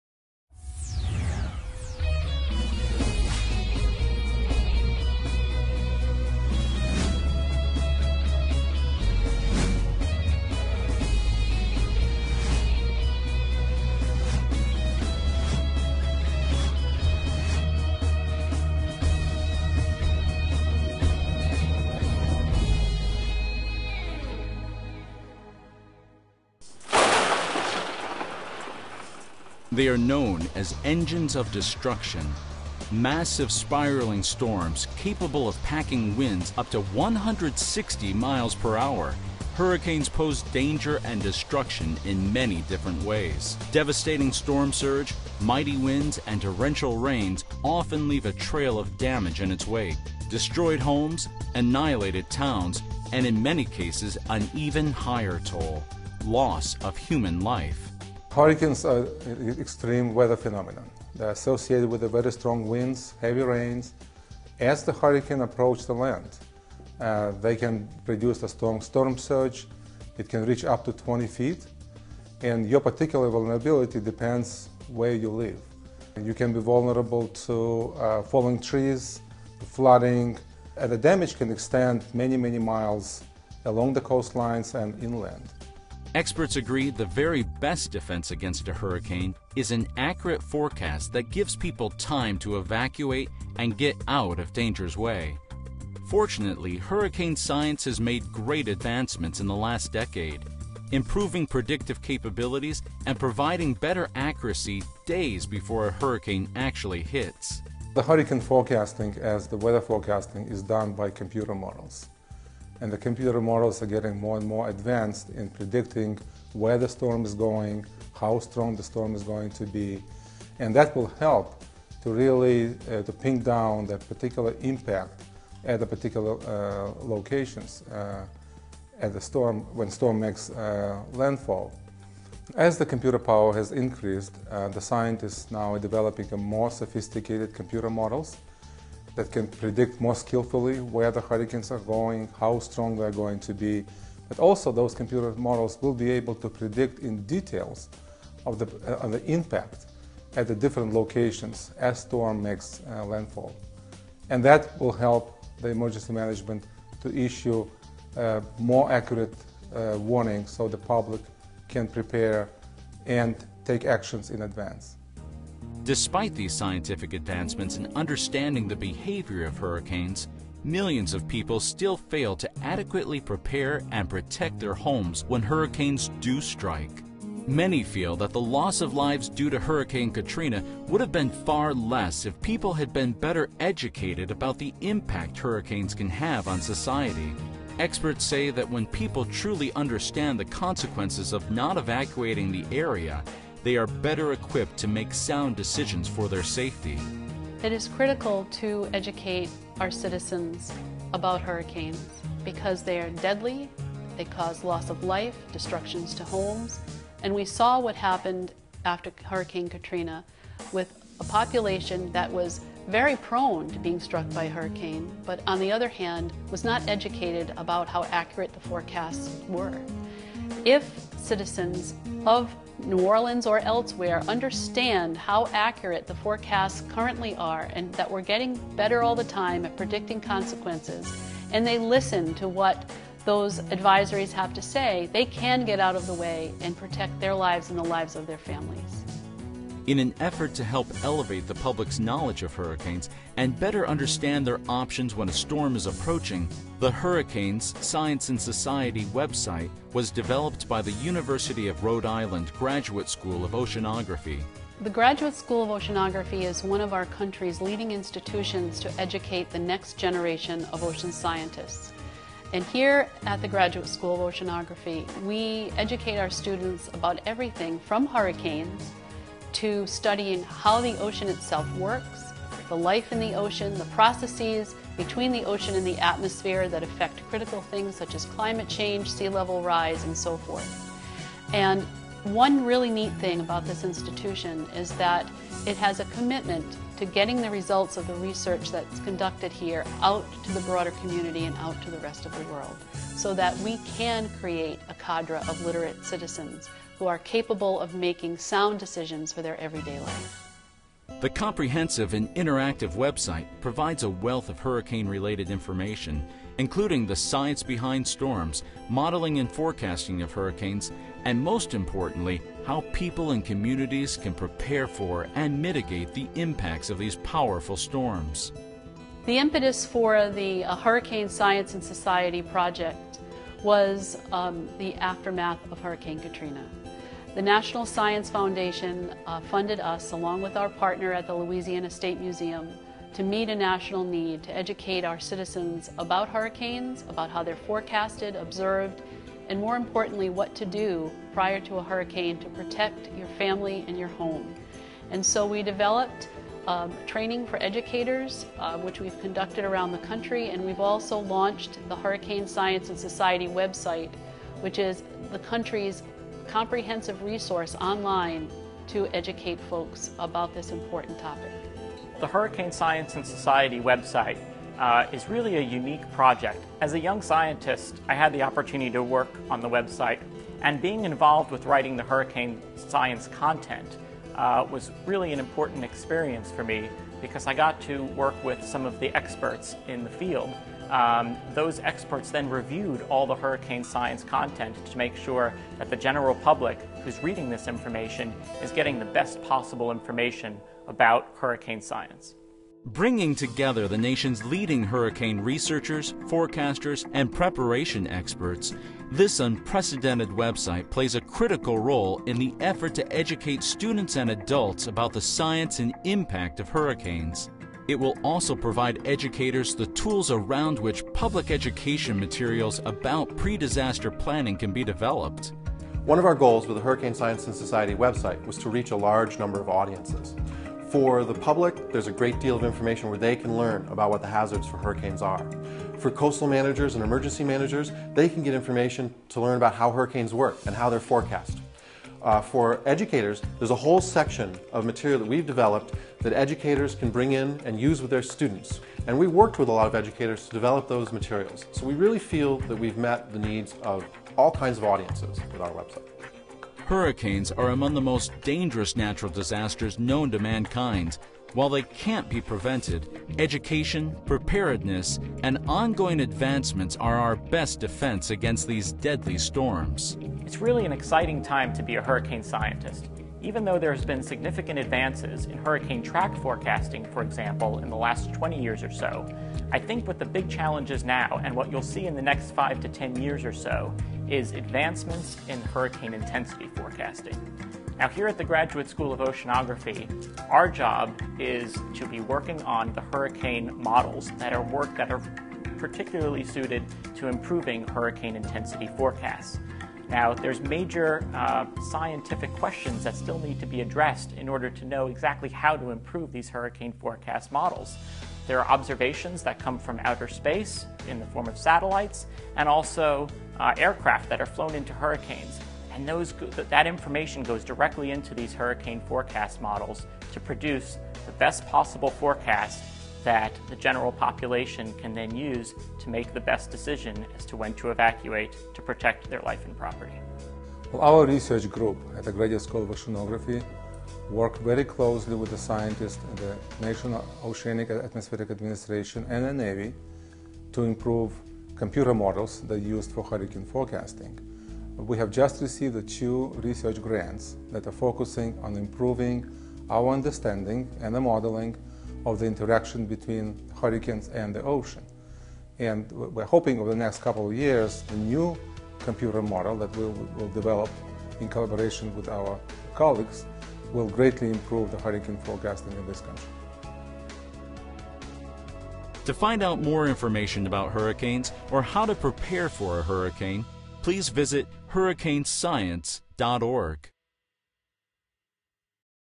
University of Rhode Island - Hurricanes: Science and Society
Major goals of Hurricanes: Science and Society are to provide foundational science for understanding complex scientific content, inform visitors about current scientific and technological advances, and to help visitors make good decisions prior to and during a hurricane emergency.

The Hurricanes: Science and Society website and associated educational resources provide information on the science of hurricanes, methods of observing hurricanes, modeling and forecasting of hurricanes, how hurricanes impact society, and how people and communities can prepare for and mitigate the impacts of hurricanes. There is also a hurricane history interactive, hurricane case studies, and a special section for educators. The information on the Hurricane: Science and Society is based solely on published scientific research and is a result of the Hurricanes: Science and Society project. All content has undergone thorough peer review by a panel of scientific experts.